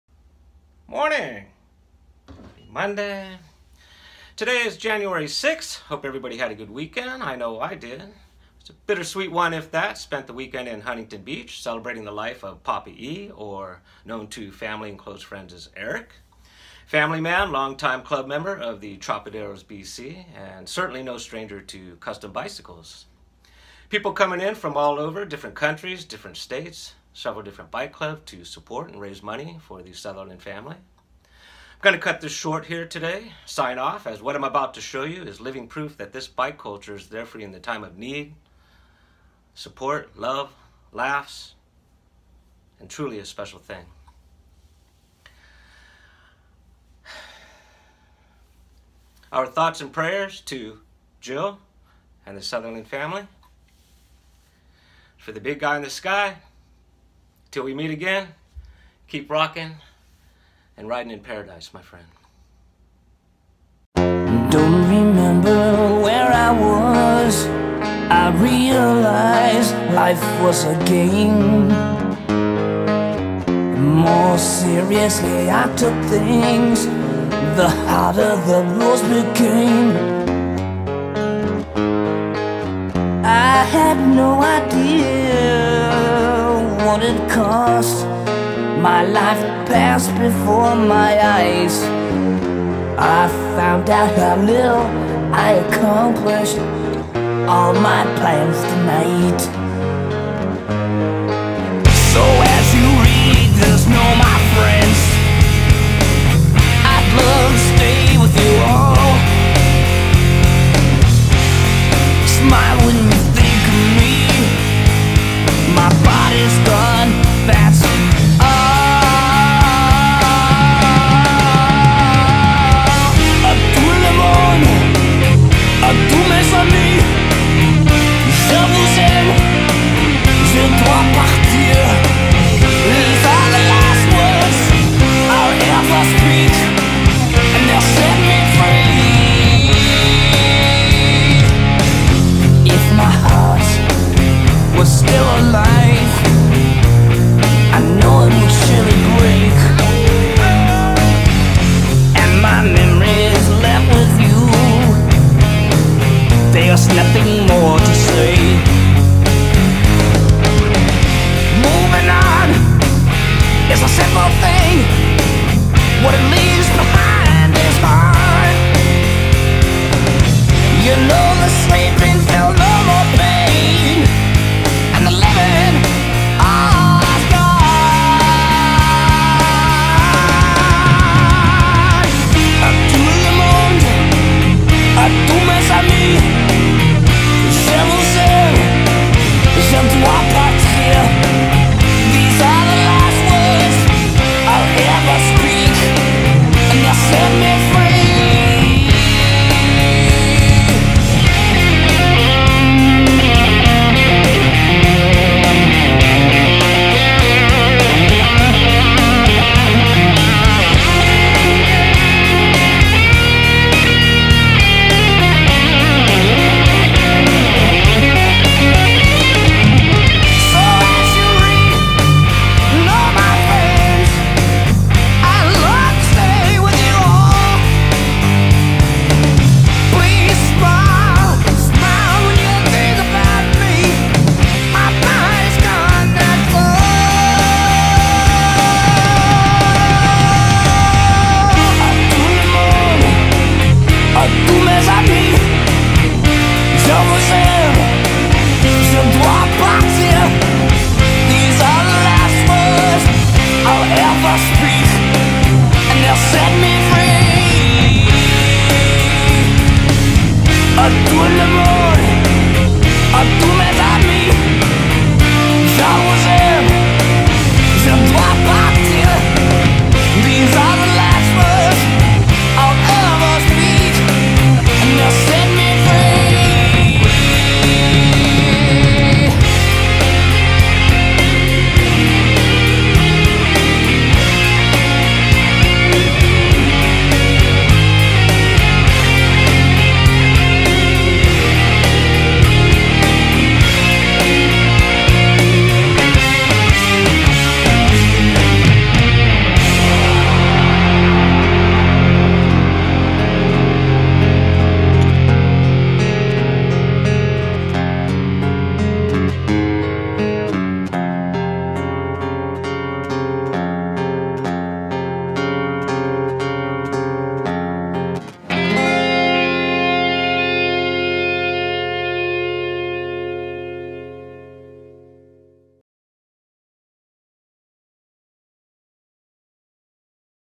Celebration of Papa E’s Life, Huntington Beach California
01/07/2020 Happy Tuesday! Today we celebrate the Life of Eric Southerland, most known as “Papa E”. A weekend in Huntington Beach California where the event kicked off with a Meet & Greet, followed by a raffle fund raiser, ending with a Memorial Ride in Eric’s honor. Love, laughter and tears was in the air of which Papa E wouldn’t have had it any other way. In Loving Memory of Eric Southerland, our thoughts and prayers to his wife Jill and the entire Southerland family.